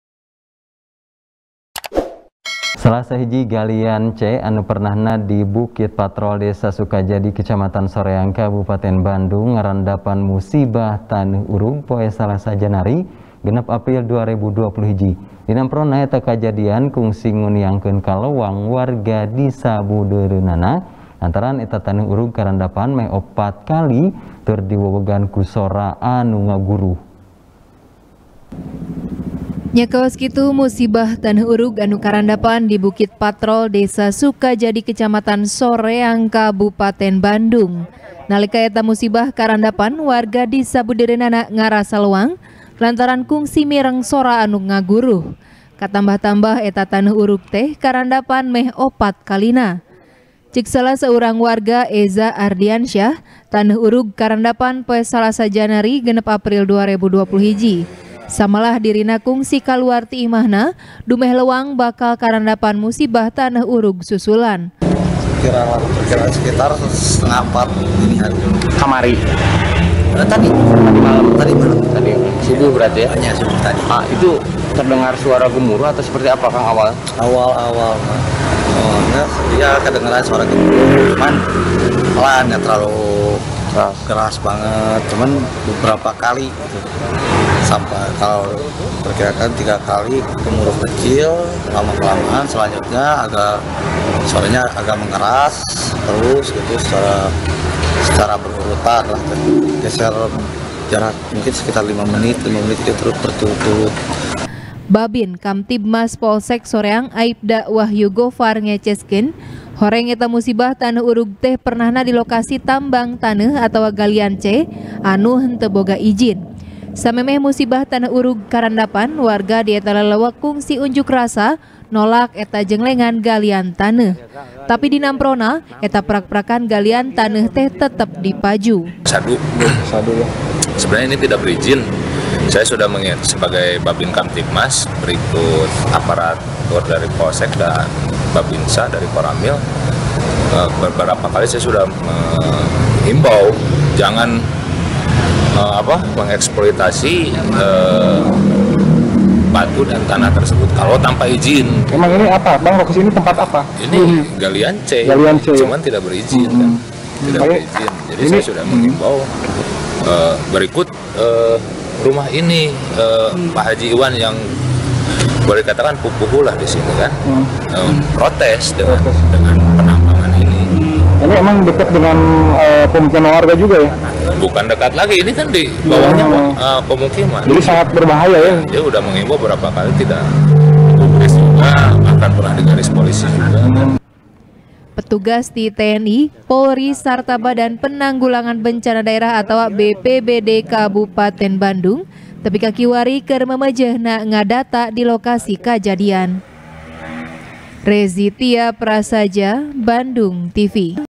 Urug, Galian C Di Bukit Patrol Soreang | TP | BANDUNGTV
TP | BANDUNGTV | IWAN M RIDWAN | Salah Sahiji Galian C Anu Pernahna Di Bukit Patrol, Desa Sukajadi, Kecamatan Soreang Kabupaten Bandung Ngarandapan Musibah Taneuh Urug, Poe Salasa Janari, 6 April 2021. Dina Emprona, Eta Kajadian Kungsi Nguniangkeun Kalewang Warga Di Sabudeureunana, Lantaran Eta Taneu Urug Karandapan Meh Opat Kali Tur Diwewegan Ku Sora Anu Ngaguruh.